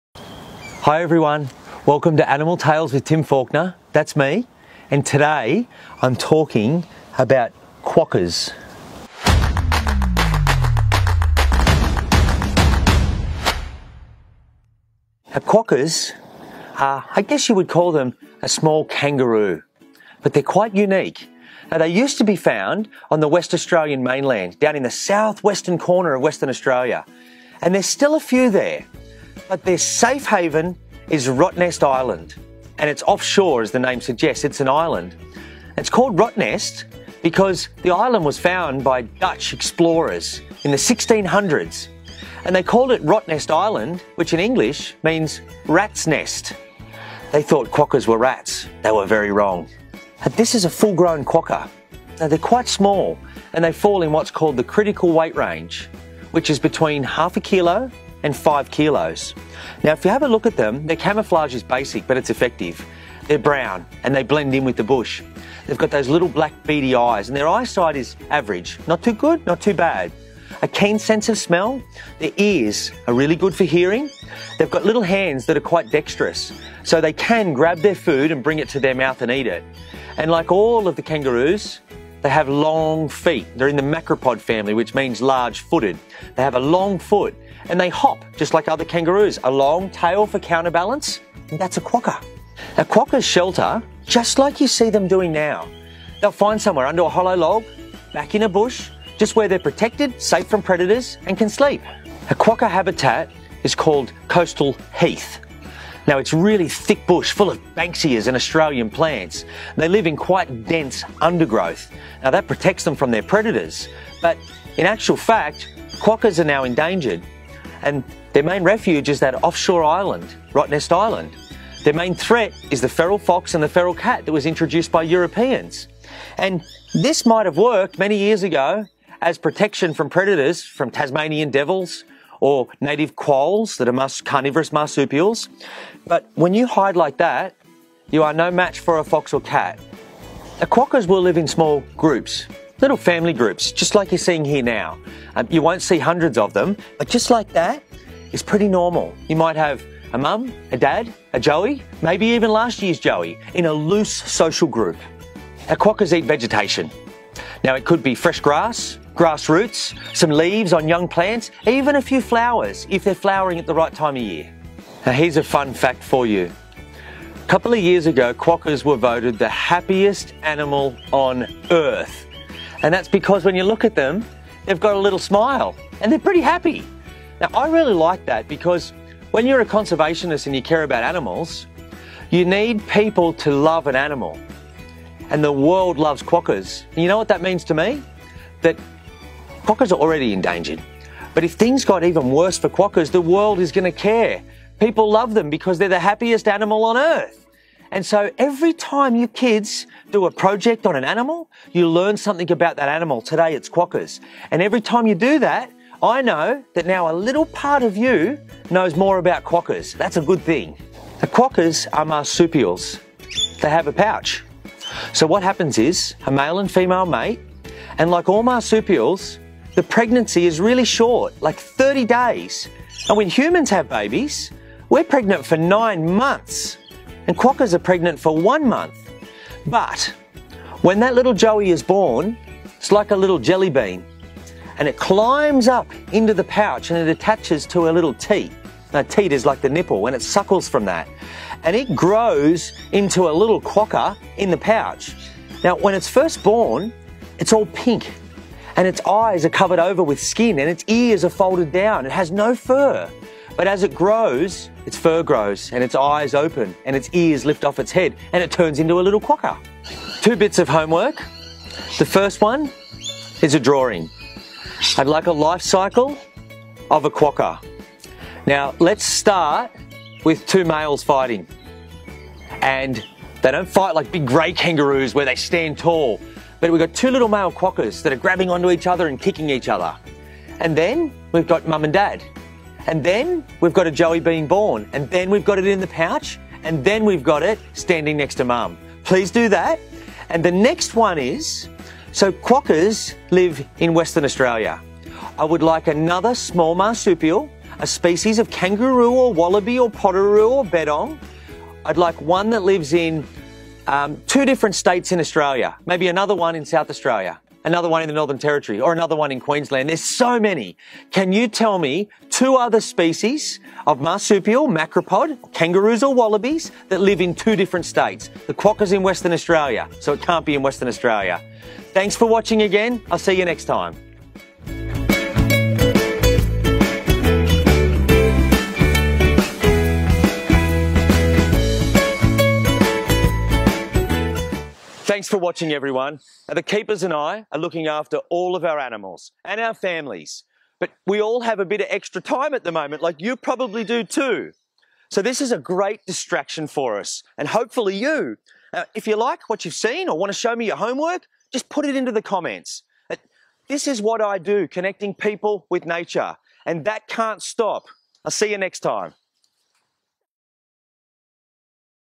ANIMAL TALES WITH TIM FAULKNER | EPISODE 16 | QUOKKAS
🐾 ANIMAL TALES WITH TIM FAULKNER! 🐾

In today's episode Tim Faulkner talks about the worlds happiest animal, the Quokka!

Animal Tales with Tim Faulkner is an educational series for students to learn about our Australian wildlife, and how you can help protect them!Don't forget to post your homework on our Facebook page! 👇